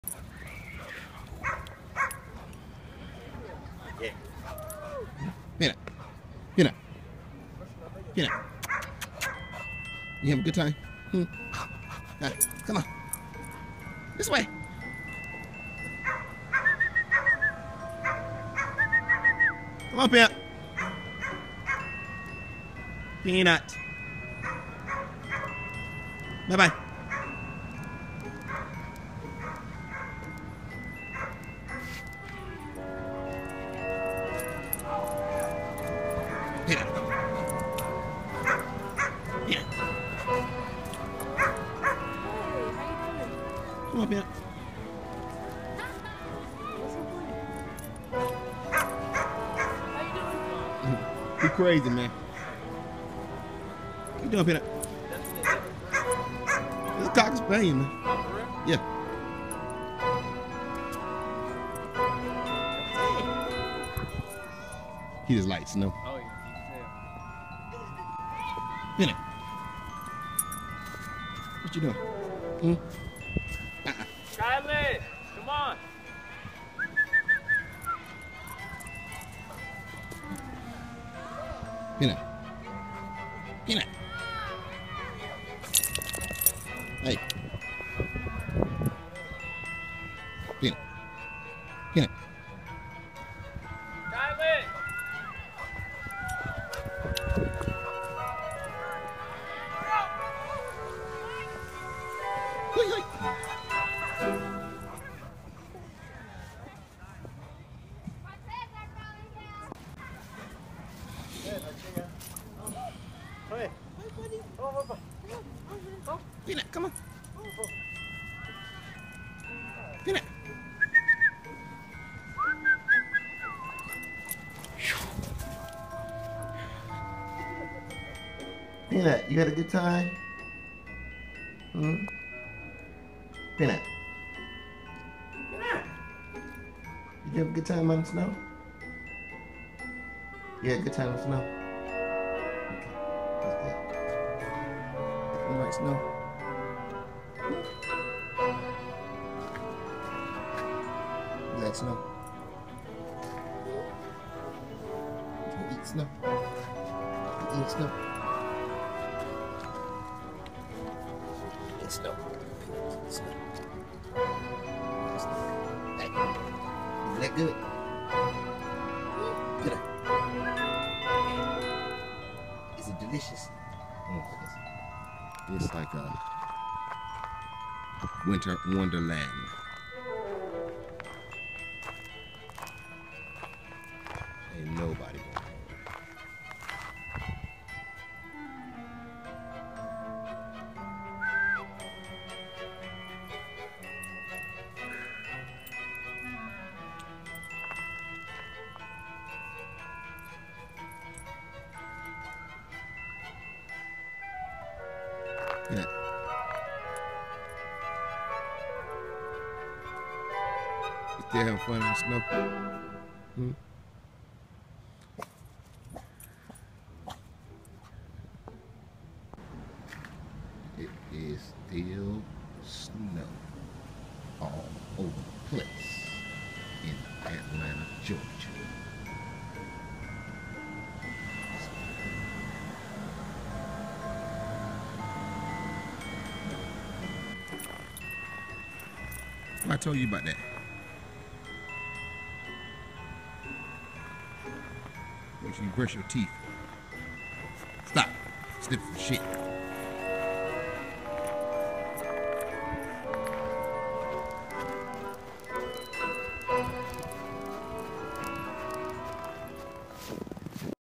Peanuts snow day in Atlanta
Peanut is just being himself only colder